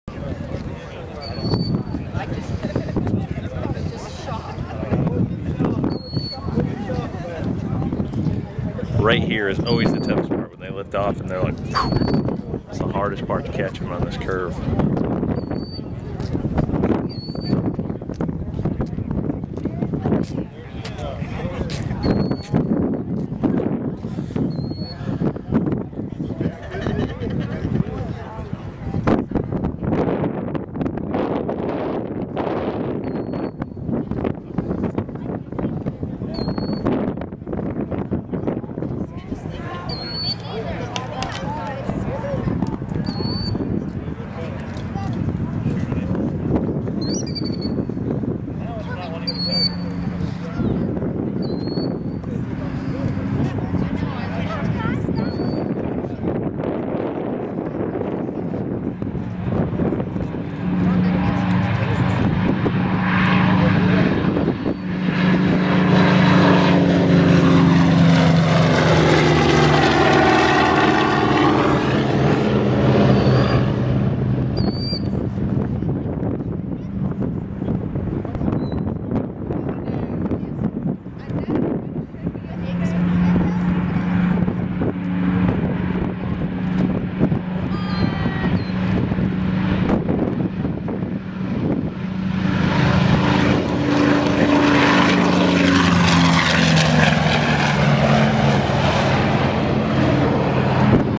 Thunderbolt Taxi & Take Off
2013 Fly in at the Tennessee Museum of Aviation Sevierville